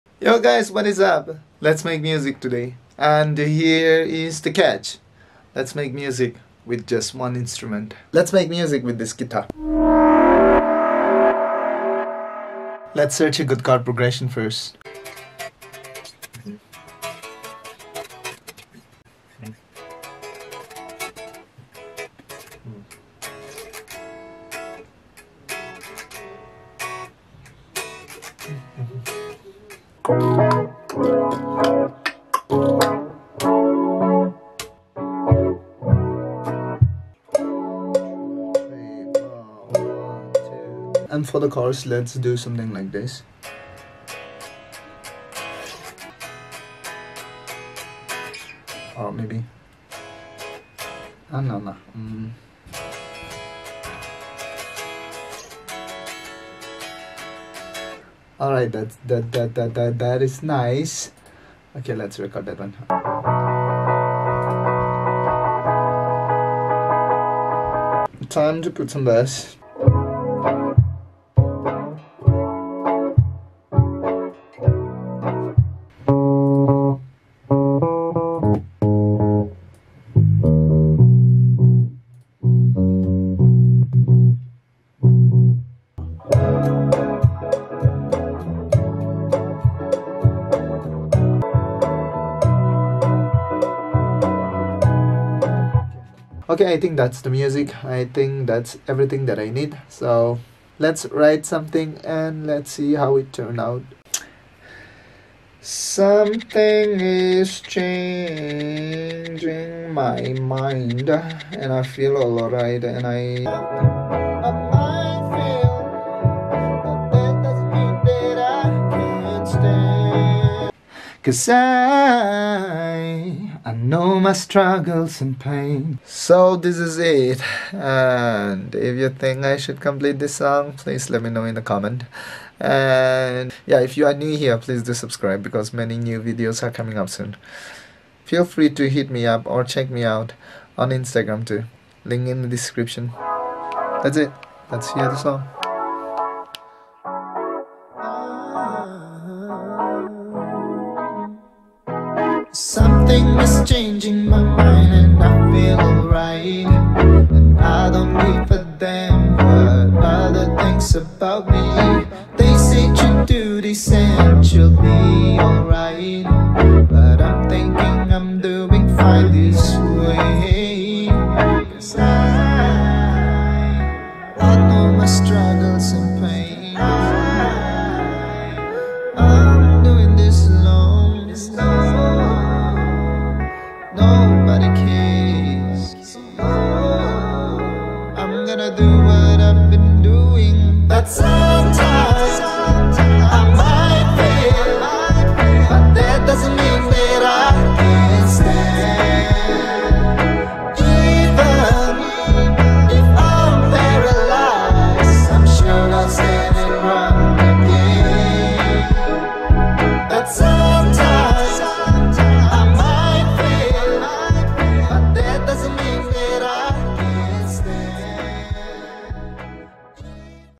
When you don't know what to upload but you really want to......!!!!!
I made this whole song only with Electric Guitar.

FL Studio
Filmora 9
Canon M50